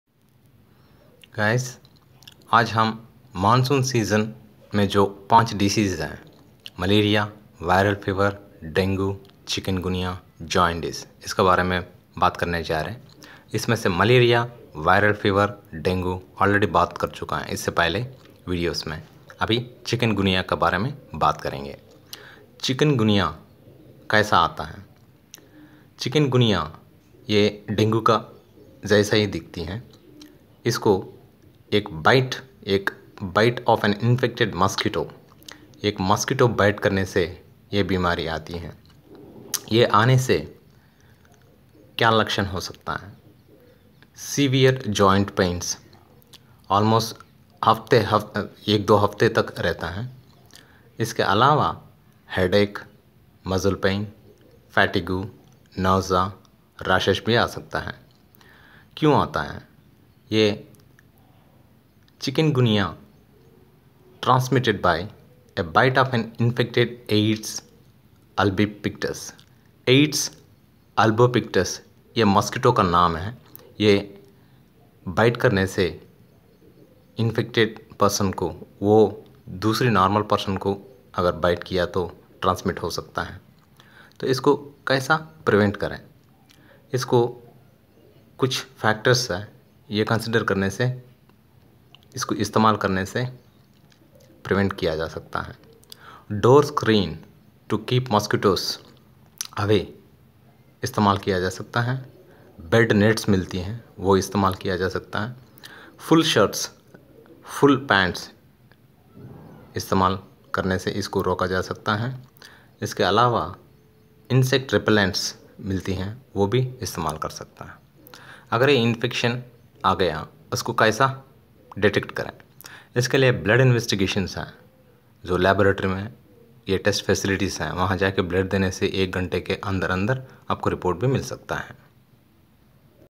Common fevers in monsoon season in Hindi: Chikungunya
Common fevers in monsoon season in Hindi by Dr Simha: chikungunya